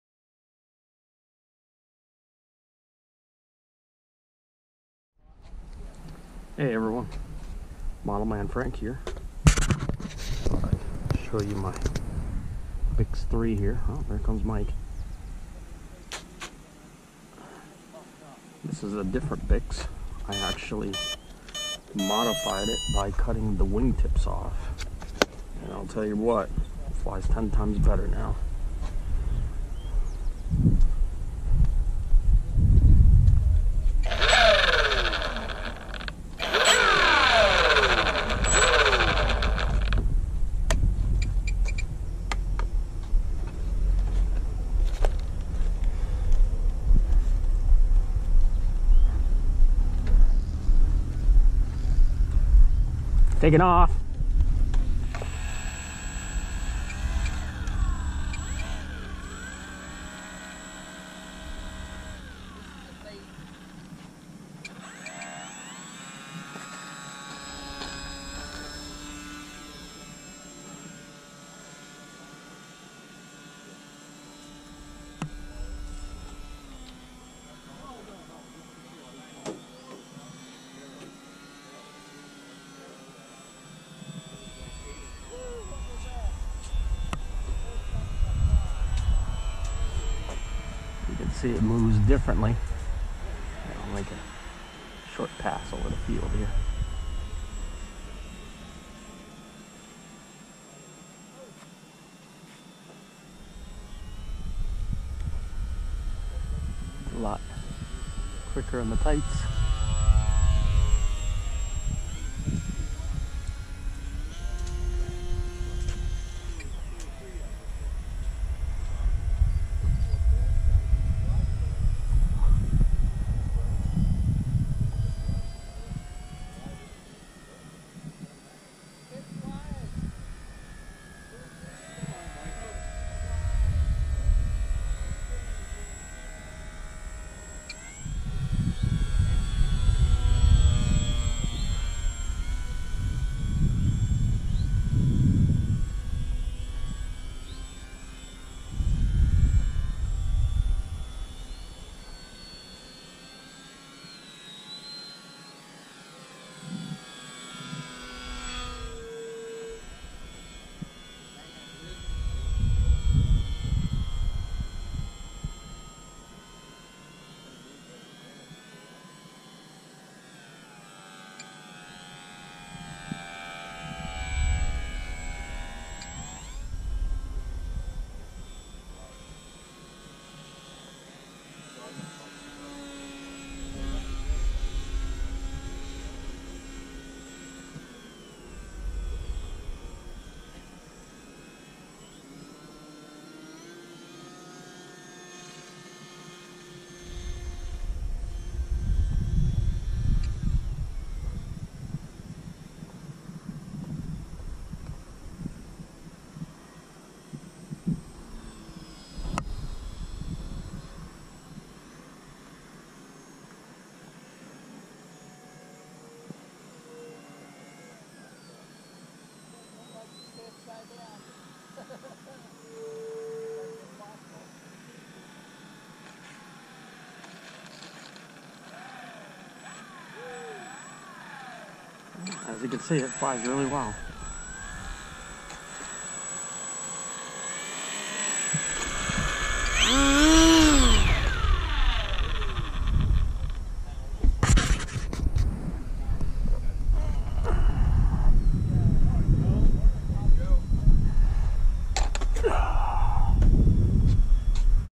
HobbyKing BiX-3 slight modified
I modified this Bix-3 I clipped the wingtips to make it more maneuverable.